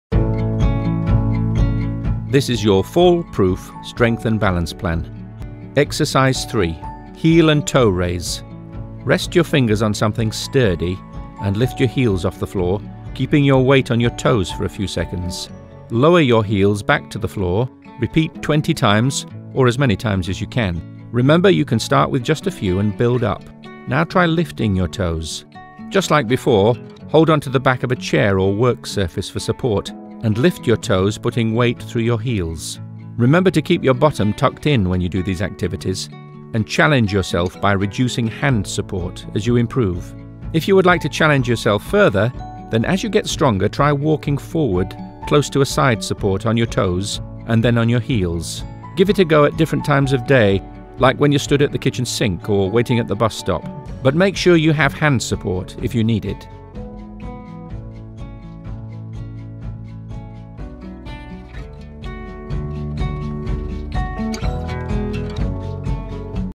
Heel and toe raise - FallProofBarnsley exercise
As people get older, it can be a little bit harder to do some of the things we take for granted doing every day. While this is a normal part of getting older, there are lots of things we can do to make sure that people over the age of 65 can do to reduce the risks of falls.

Staying active can help to improve strength and balance. Check out this playlist for simple exercises you can do around the home.

Don't forget to find out more about falls and how you can prevent them